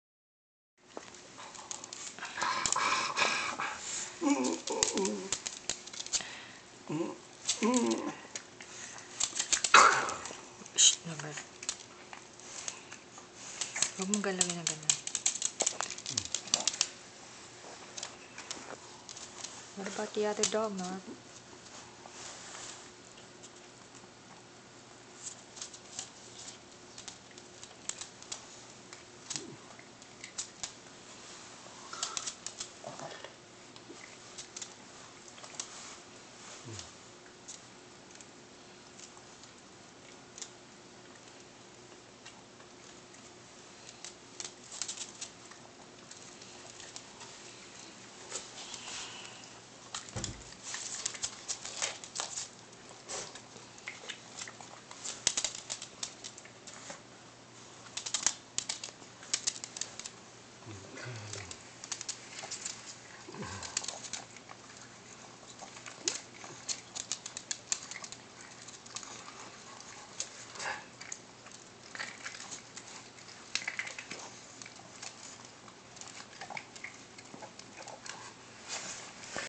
Lucille's Ribs Eaten by Chihuahua
My Chihuahua's eating a hot Rib